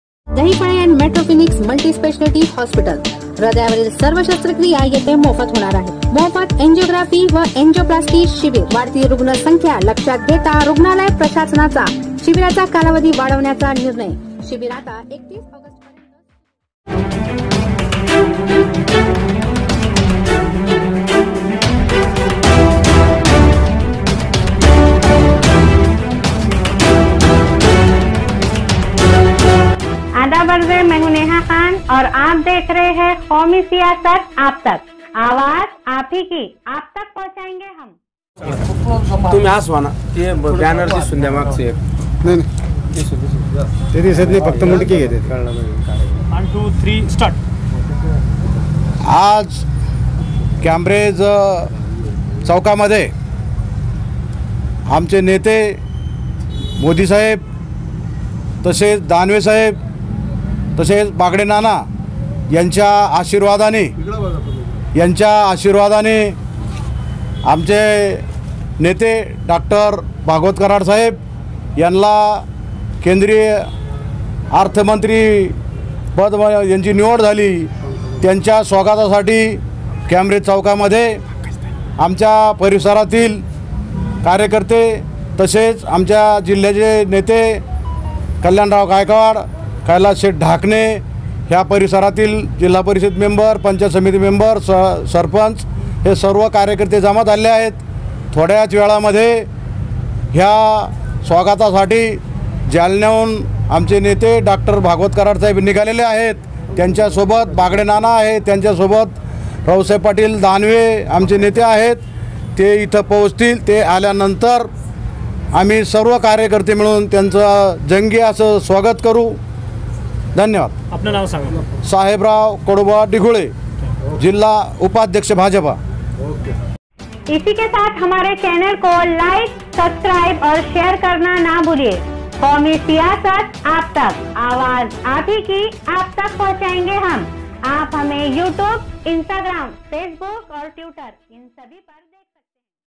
केंद्रीय मंत्री डॉ. भागवत कराड के आगमन पर स्वागत समारोह रखा साहेबराव कडुबा डिगोळे ने
Qaumi Siyasat Aap Tak